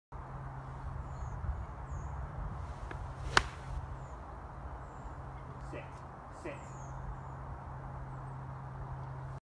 10th hole Romsey GC
PW into par 3 10th at Romsey GC